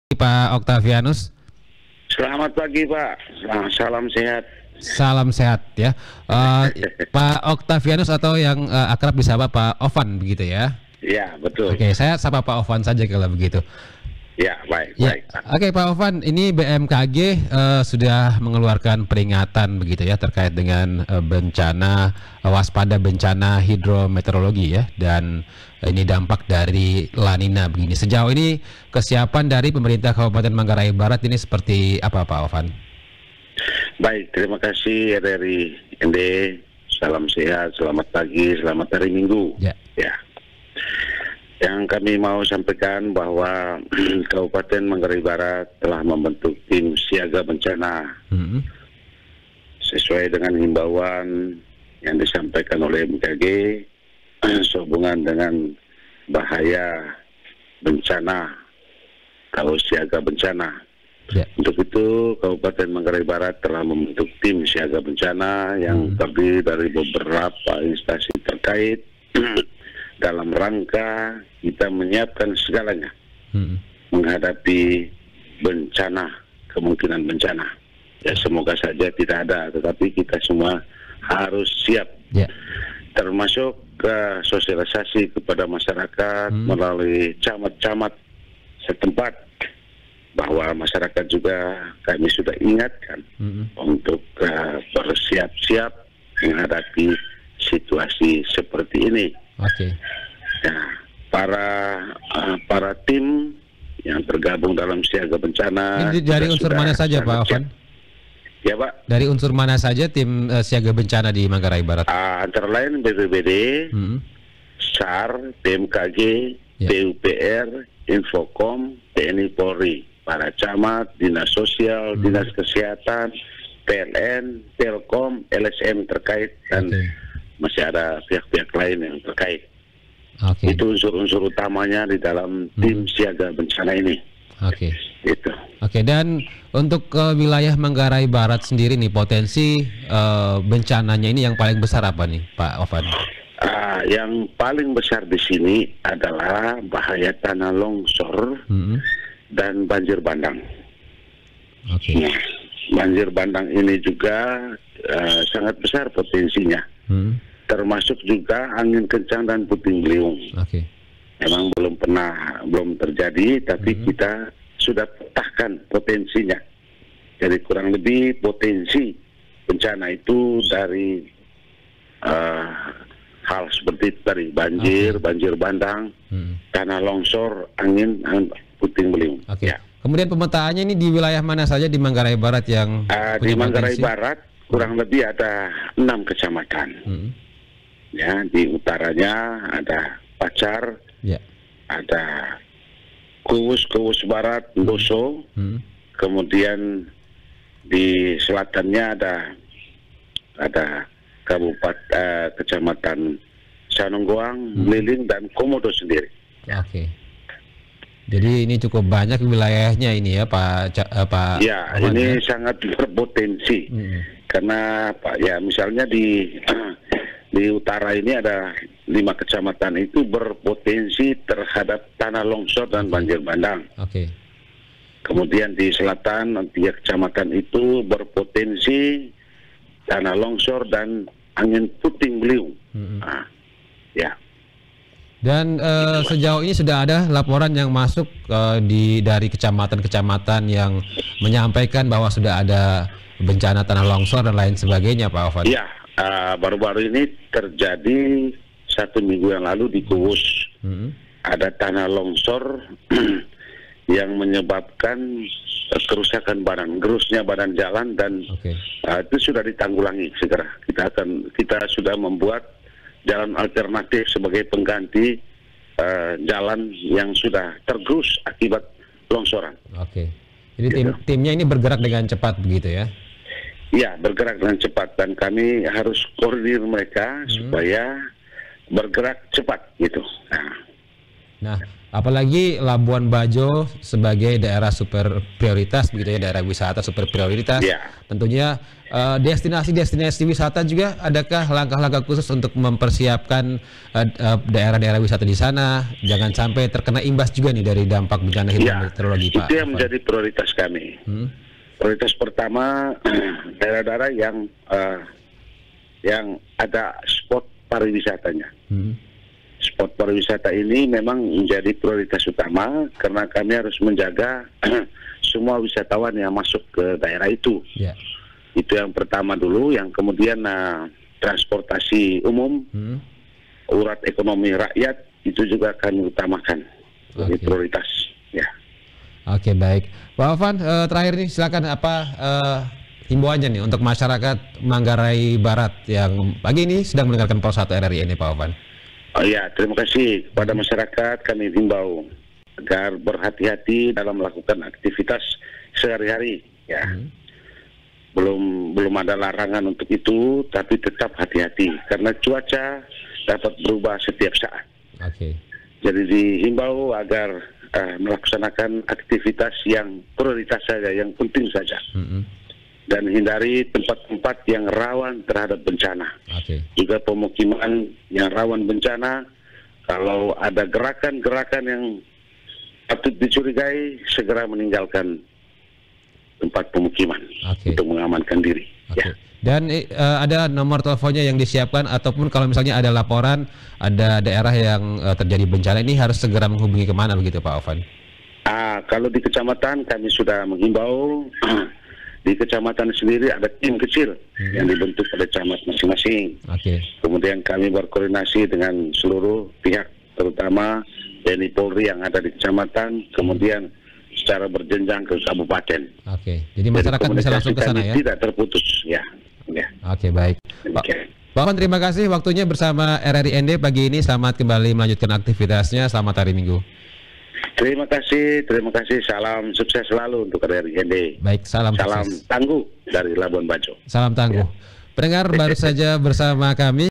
Pemkab Manggarai Barat Bentuk Tim Siaga Bencana
Narasumber :
Oktavianus Adu (Kalak BPBD Manggarai Barat)

Presenter : Riswan Sumby
Pengarah Acara : Anthonia Nona
Operator : Mikael Sega